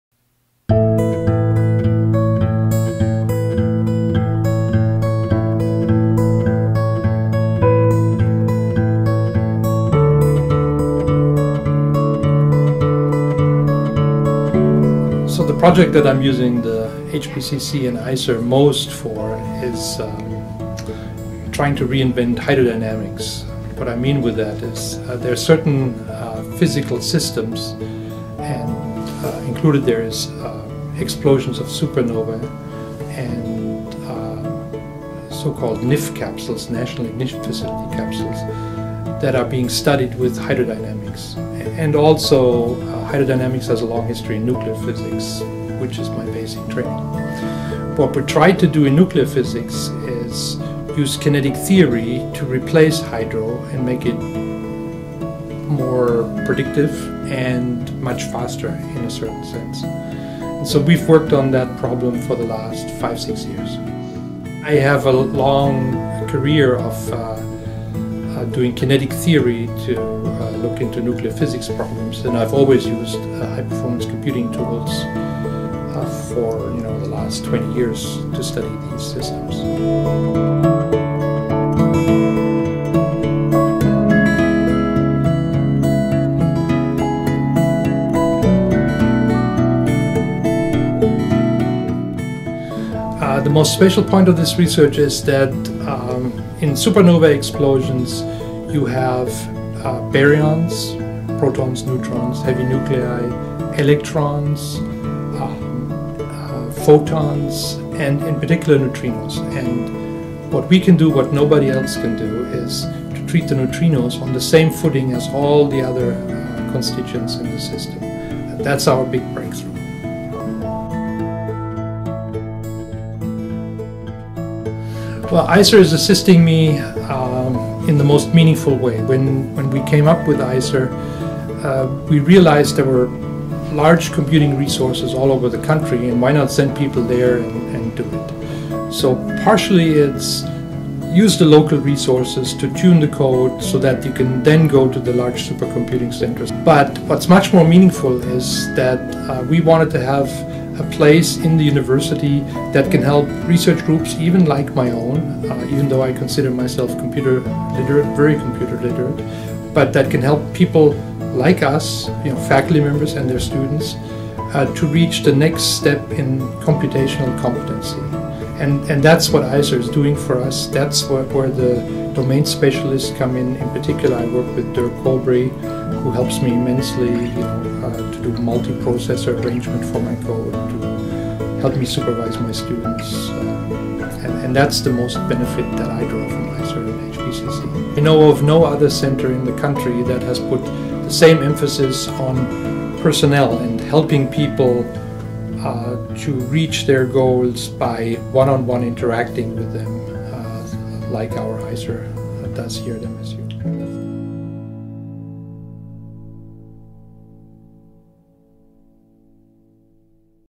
iCER research highlights - Wolfgang Bauer
Two of the most prominent ways to model the time evolution of complex many-body systems are hydrodynamics and kinetic theory.  Over the last two decades, MSU University Distinguished Professor Wolfgang Bauer has made several contributions to the kinetic theory based description of subatomic reactions and has advanced our understanding of experiment observables in high and medium energy heavy ion reactions.  In particular, his work has led to a deeper understanding of the nuclear equation of state and phase transitions of nuclear matter in finite size systems.

Now Prof. Bauer is applying his techniques and computer models to systems of vastly different length, mass, and time scales: the supernova explosions of massive stars at the end of their live.  Modeling supernova explosions has long been the domain of hydrodynamics.  However, these explosions are, to a large degree, driven by neutrinos.  These neutrinos can have very long mean free paths due to their very small interaction probabilities and are thus not in the domain of validity of hydrodynamics.  The conventional way to remedy this shortcoming is to couple hydrodynamics to a neutrino Boltzmann transport calculation.  Bauer's answer is much more straightforward: apply kinetic theory, which is free of assumptions about the mean free paths, to all matter constituents, baryons as well as neutrinos.  In his approach, he and his research group simultaneously solve billions of coupled first-order differential equations in time for so-called testparticles, which makes their work an ideal candidate for massively parallel multi-processor environments such as those provided by iCER's High Performance Computer Center.